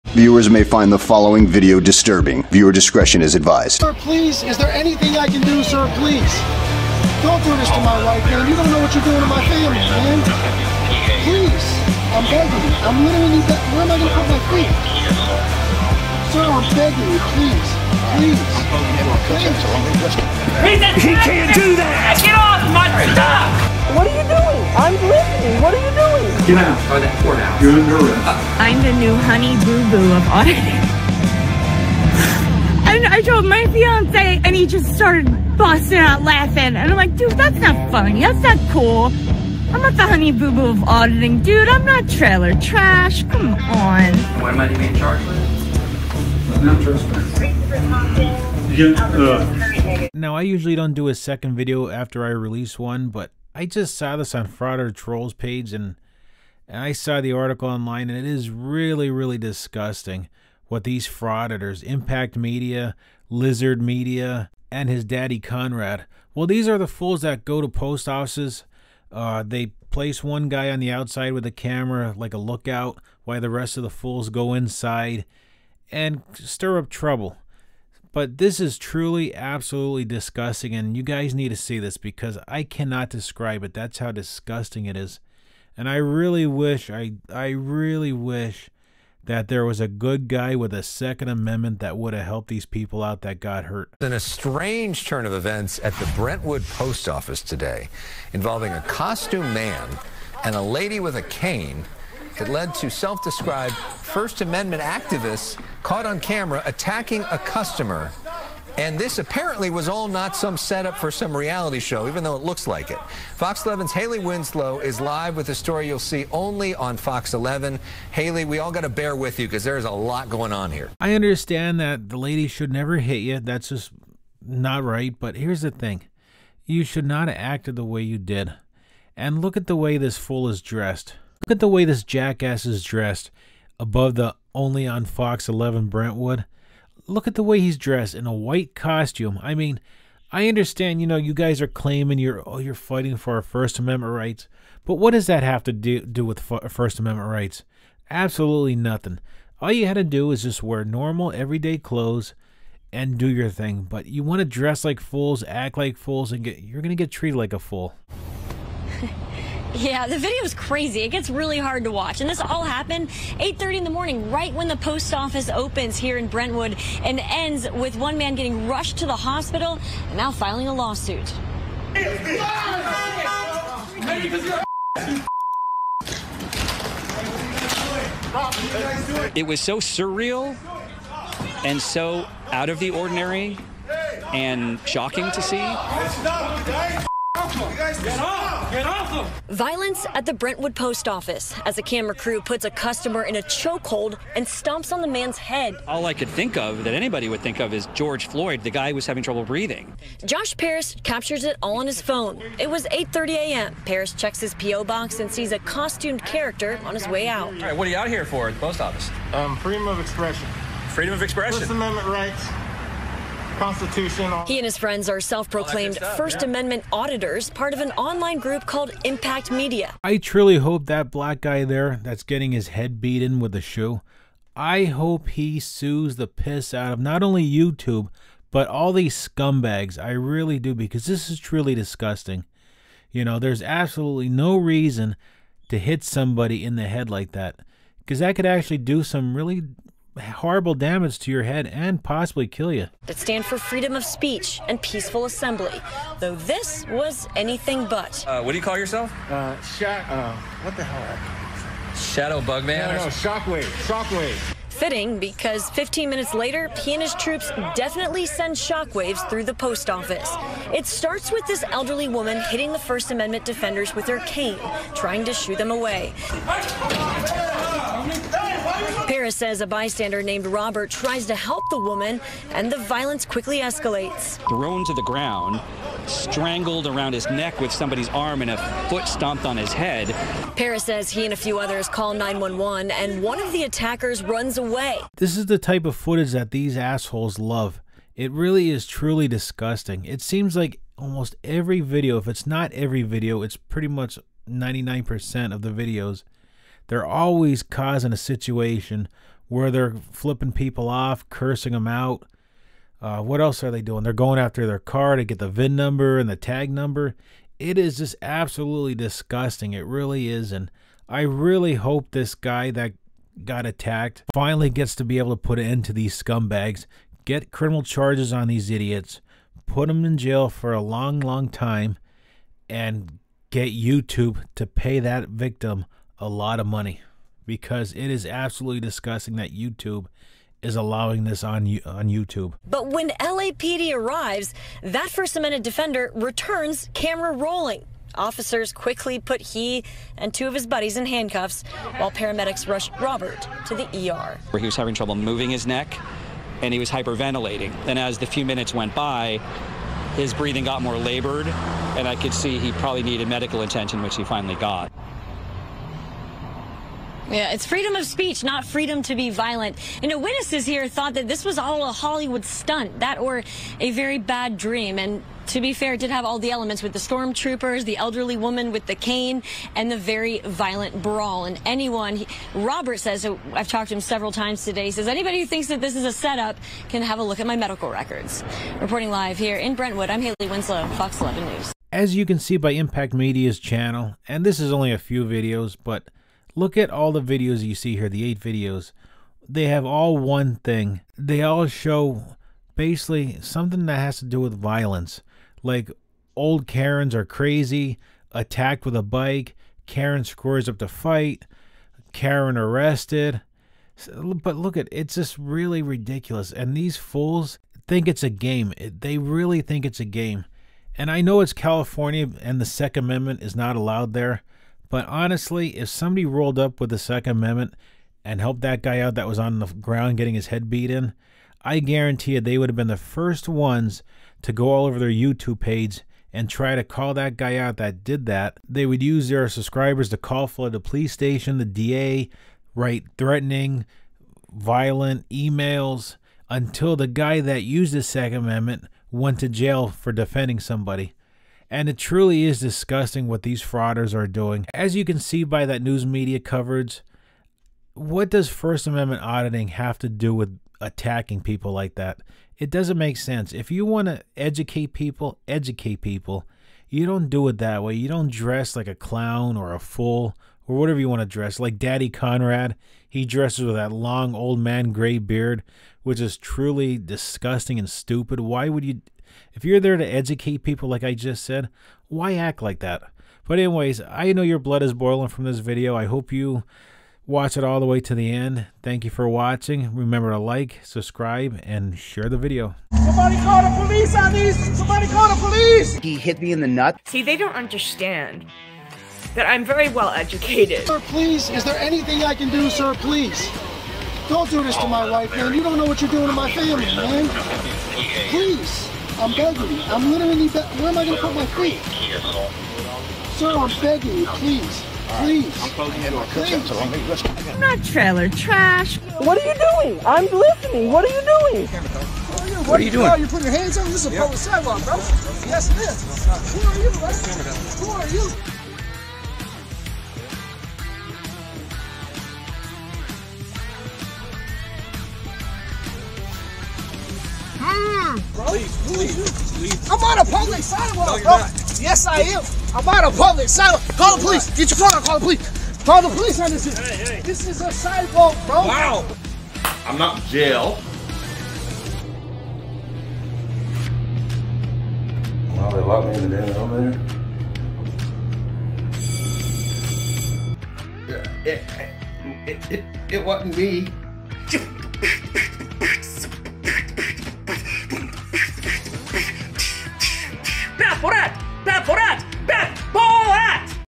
Low Life Frauditors Arrested After Attacking Senior Citizens WTH!!!
Join for Exclusive Perks: Troll Media Group Memberships Now Available!

Enjoy the latest video with special access!

🔗 Join Now:    / @trollmediagroup

Video starts on/around 1 minute 20 seconds.

👮♂️ Categories: Frauditors, Police Videos, Police Body Cam, 1st Amendment, 1st Amendment Audit.